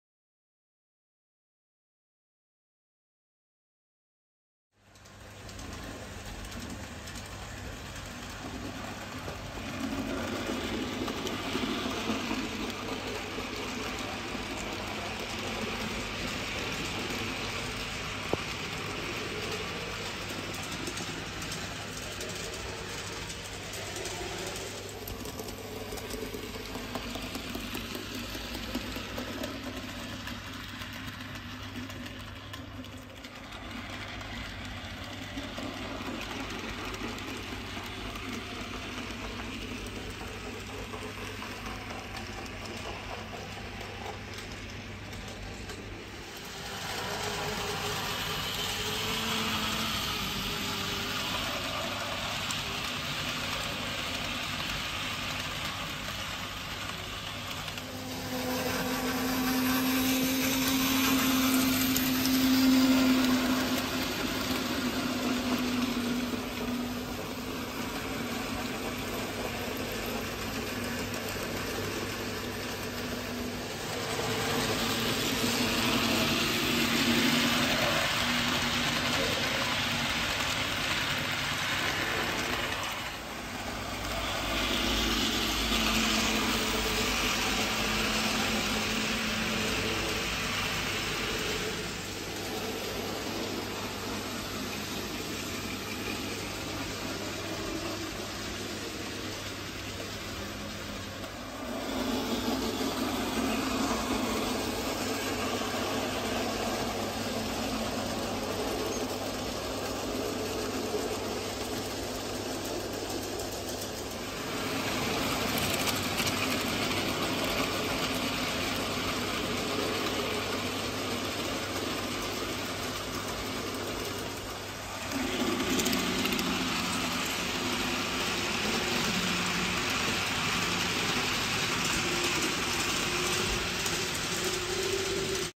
Beside the sea (model railway)
The section of sea wall on my N gauge layout "Dawlish Warren"